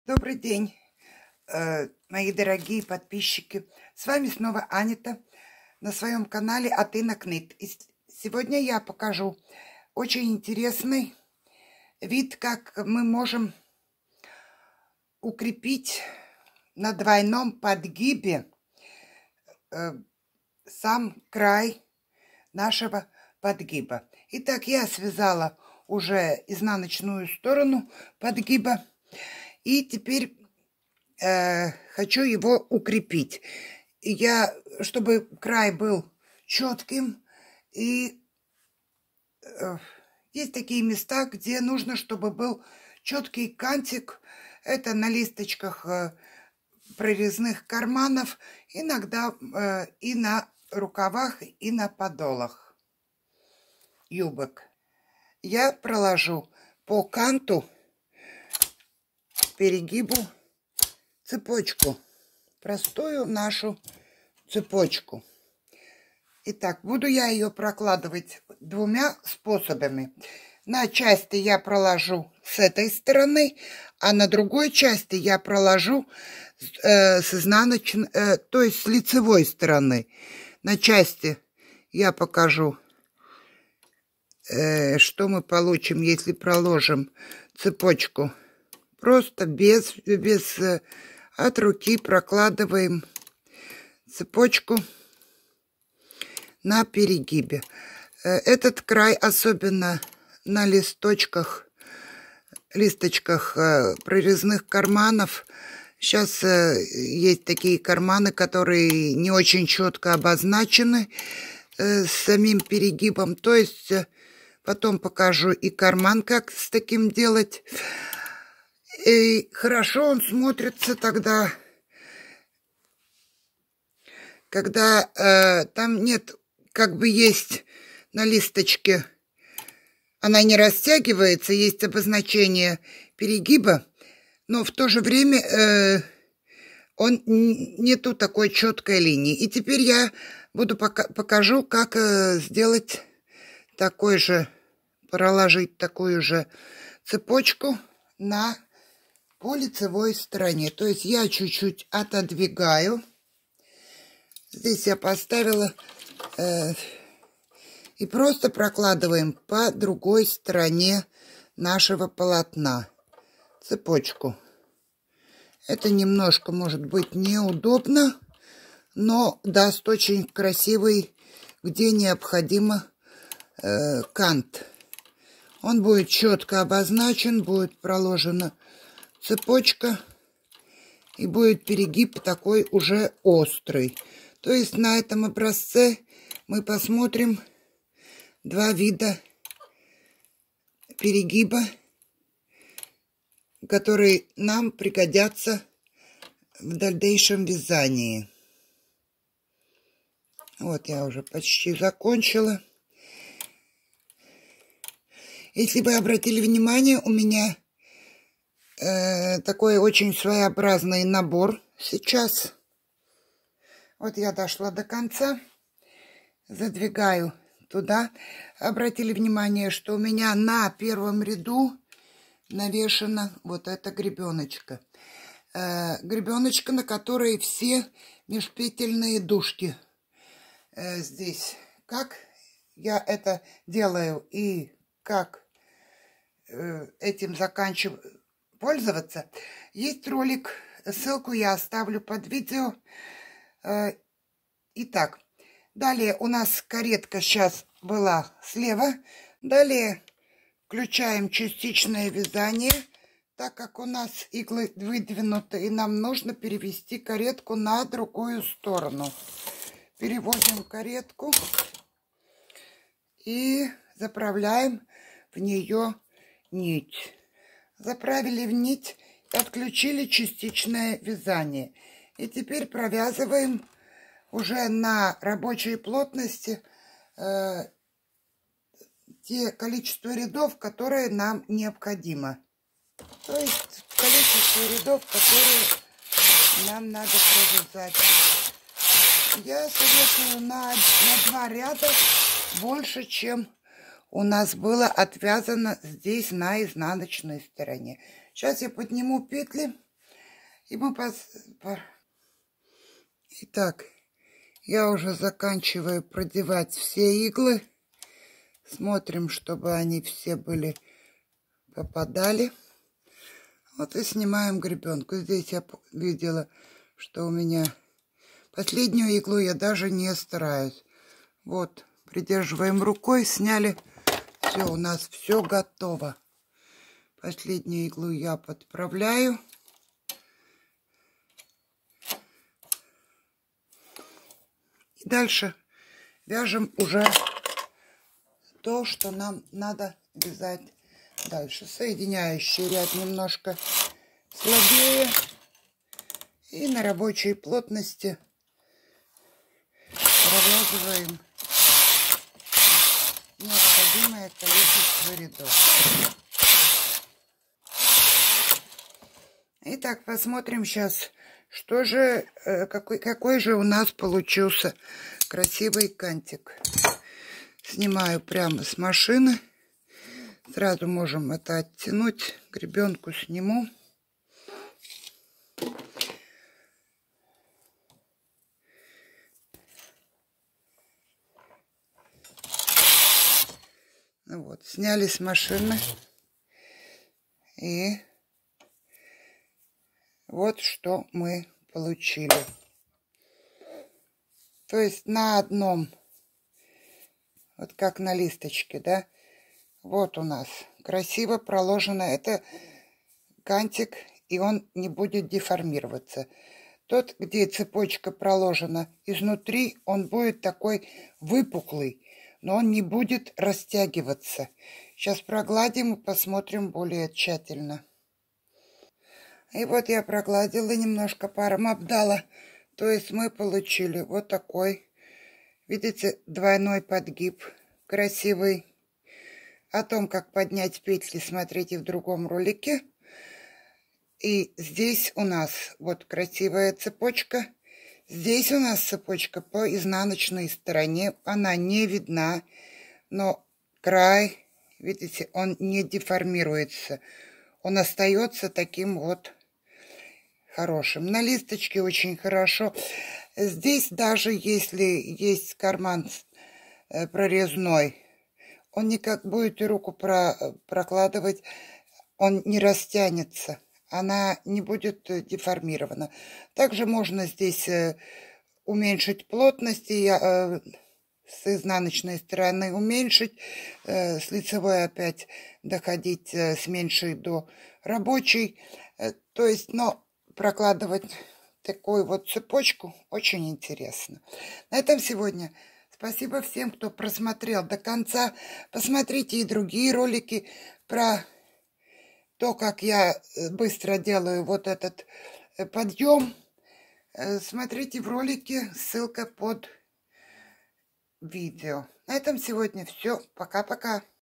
Необычная отделка перегиба листочки кармана или подгиба
Растягиваются листочки прорезных карманов? Фалдит подол юбки или платья!  надо укрепить перегиб ...цепочкой!